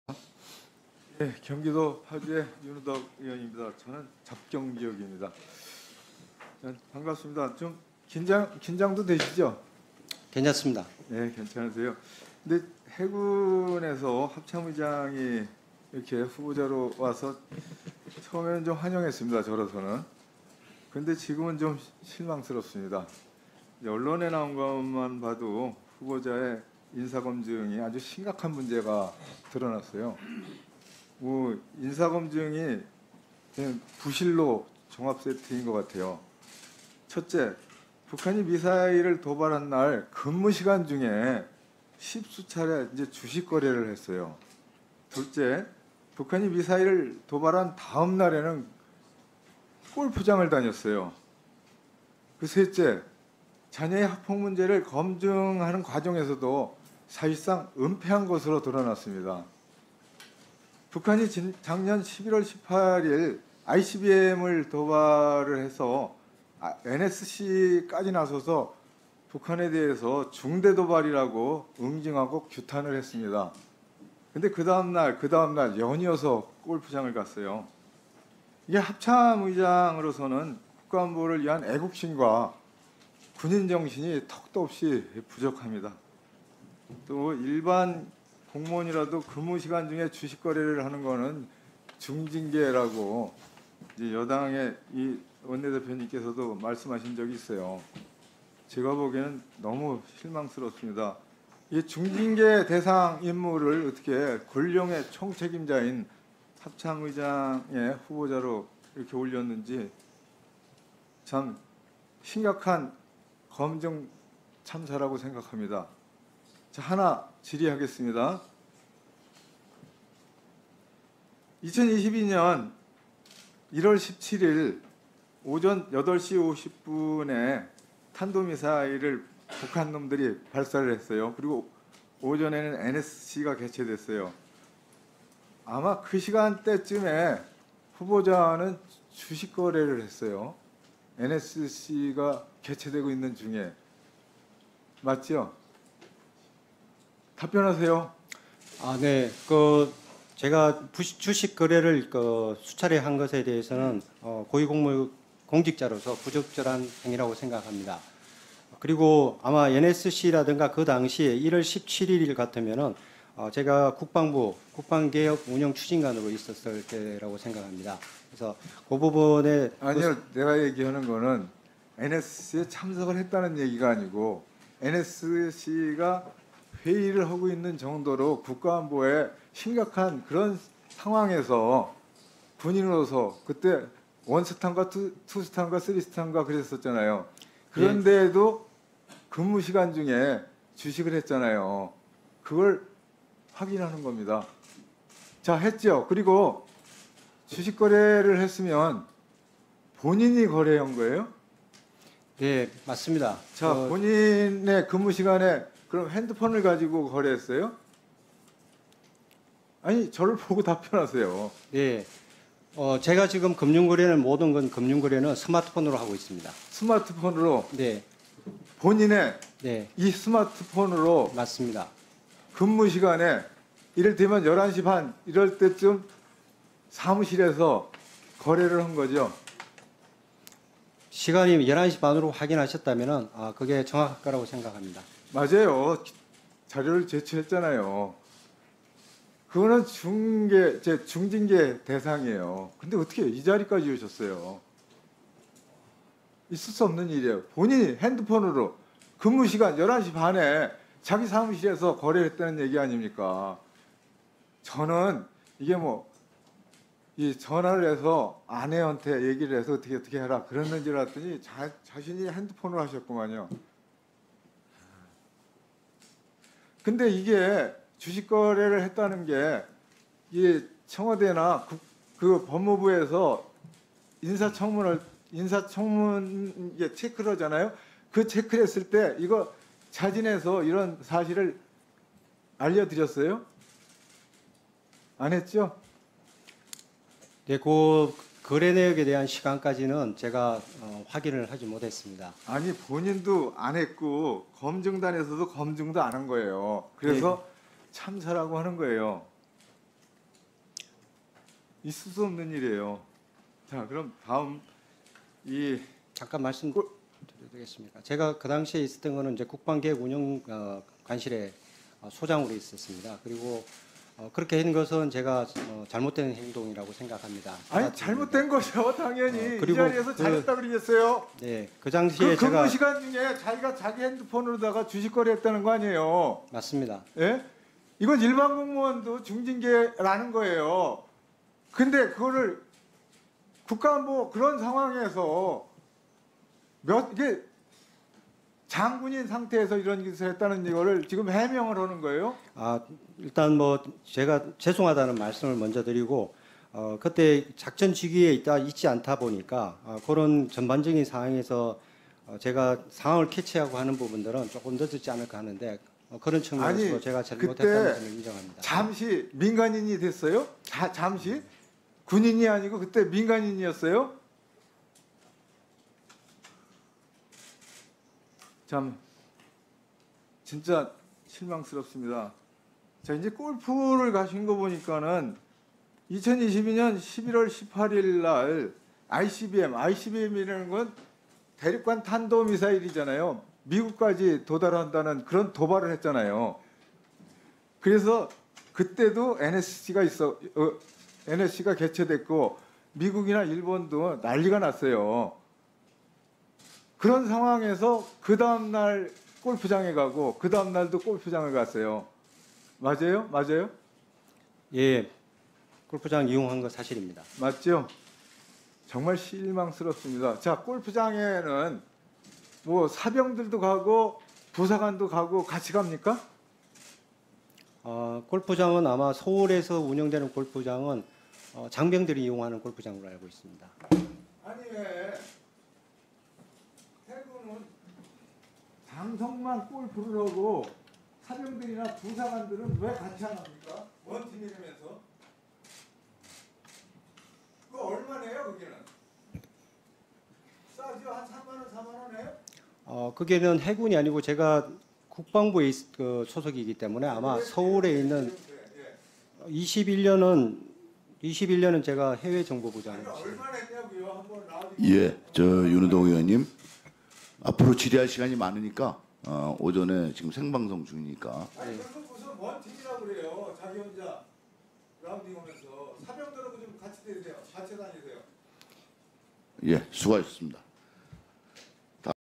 2023.11.15일 합참의장 인사청문회 더불어민주당 경기 파주시갑 국회의원 윤후덕 질의~
자녀학폭.근무시간주식거래 검증 안하고 와서 앉아 있냐~